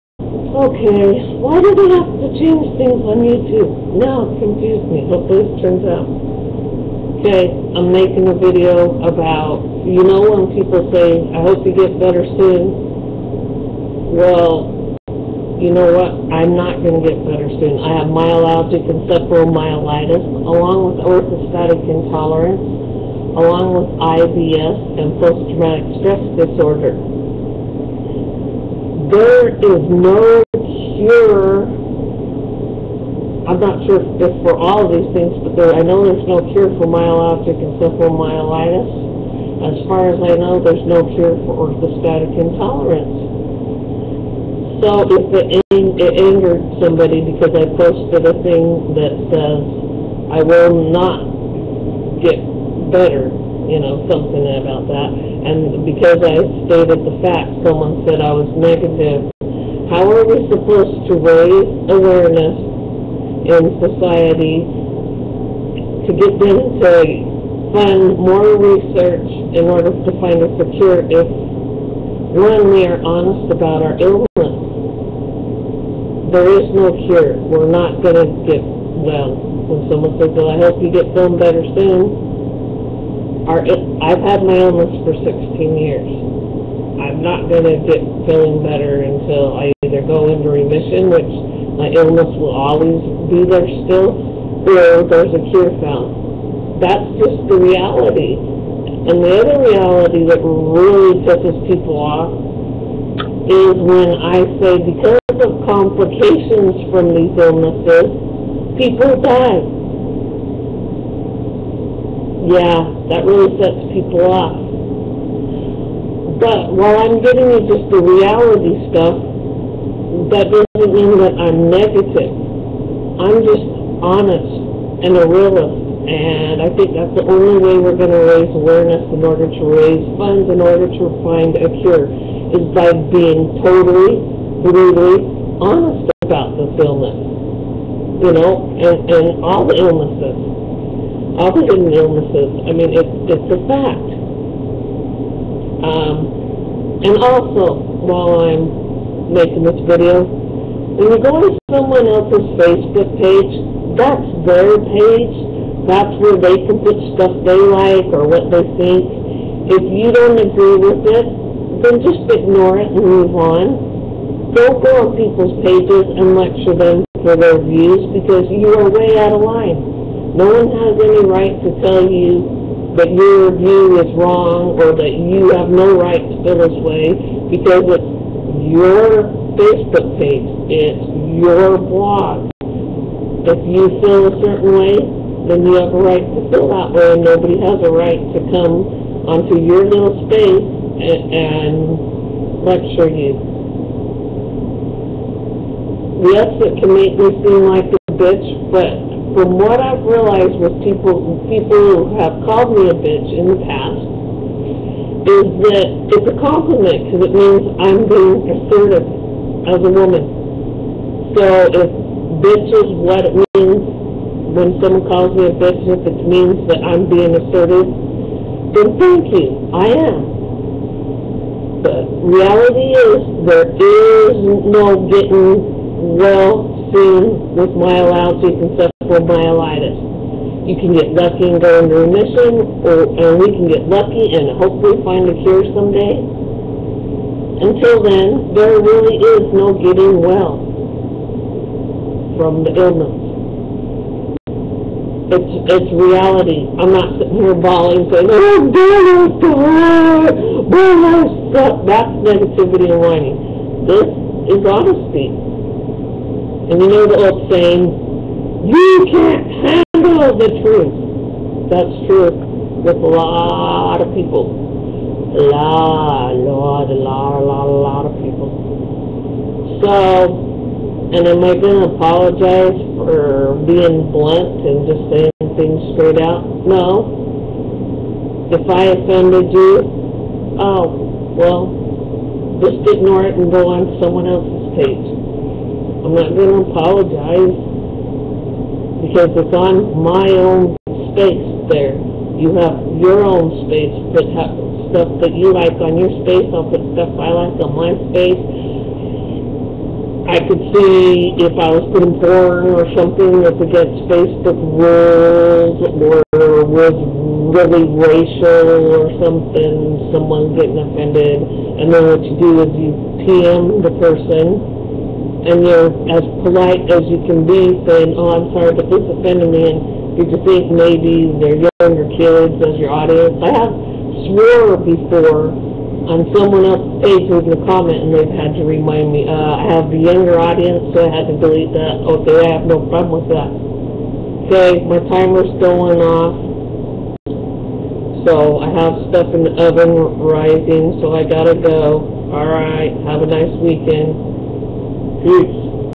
You Can't Handle The Truth & Other Truths!
The truth about ME and other hidden illnesses.I have had M.E./CFS for 16 yrs now. The sound is not right because they changed the YT settings! argh!